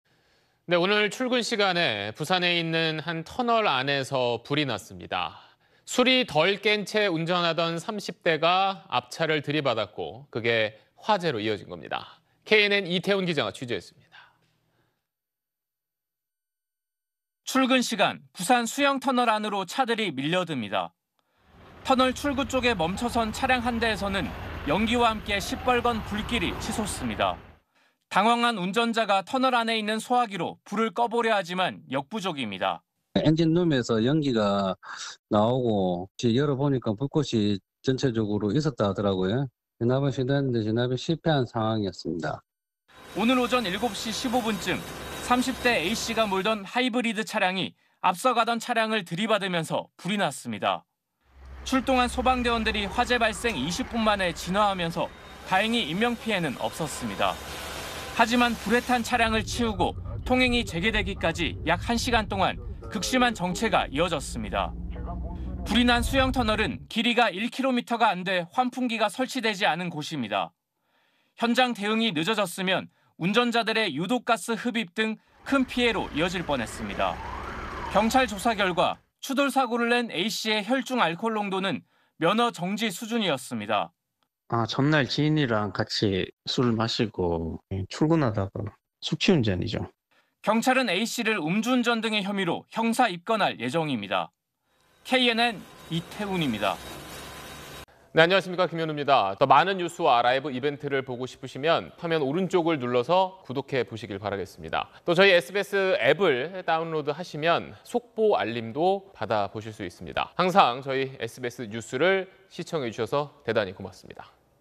술 덜 깬 채 운전하다 '쾅'…터널 차량 추돌 화재 / SBS
오늘(6일) 출근 시간에 부산에 있는 한 터널 안에서 불이 났습니다. 술이 덜 깬 채 운전하던 30대가 앞차를 들이받았고 그것이 화재로 이어진 것입니다.
KNN 이태훈 기자가 취재했습니다.